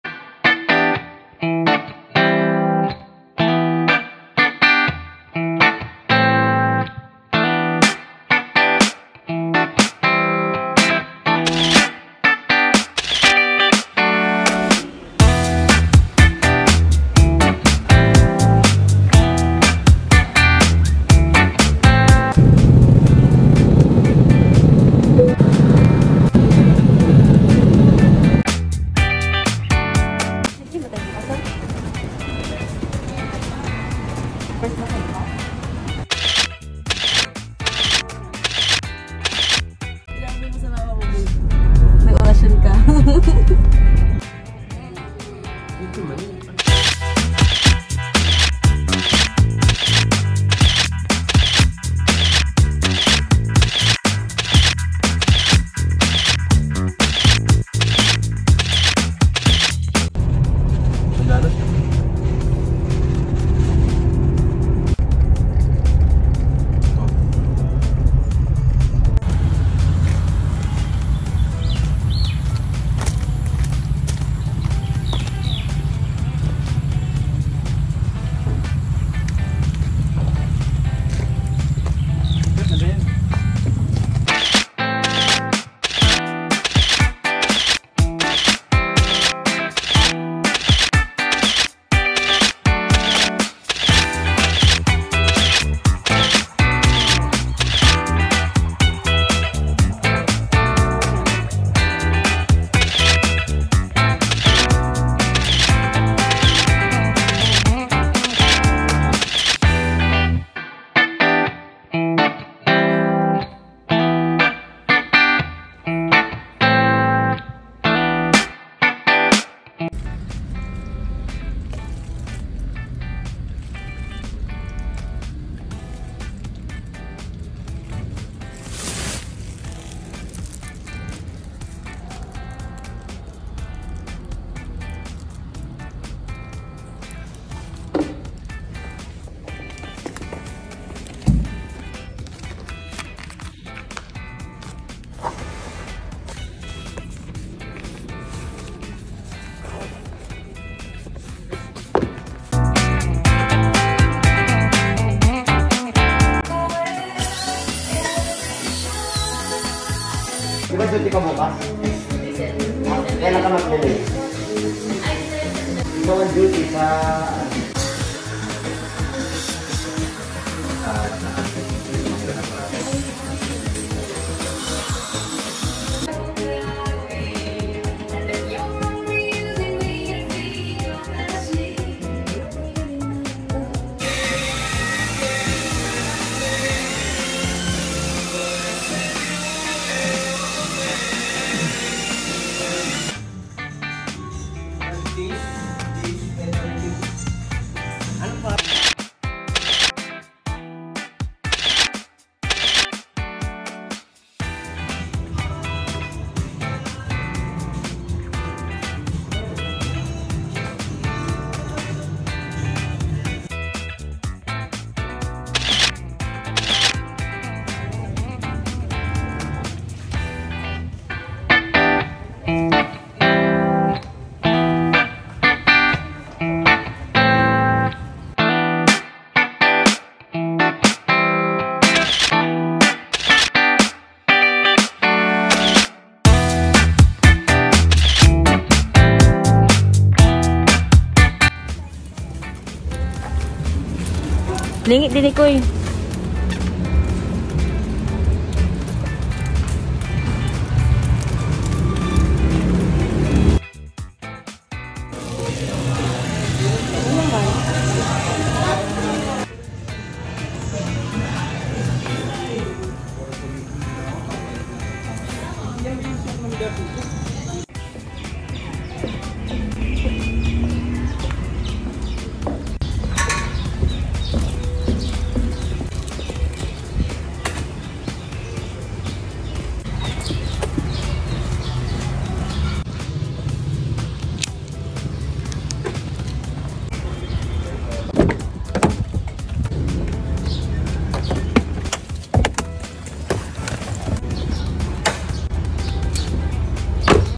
BIRTHDAY TRAVEL (2022)
Pre Birthday Travel Treat by Sister Ernesr Marie Jane Laplana.
CDO-MANILA-NUEVA ECIJA-FORTH MAGSAYSAY-CABANATUAN CITY-GAPAN CITY-MANILA-CDO

Video Edit by: Cullen Jay
Music Edit by: Cullen Jay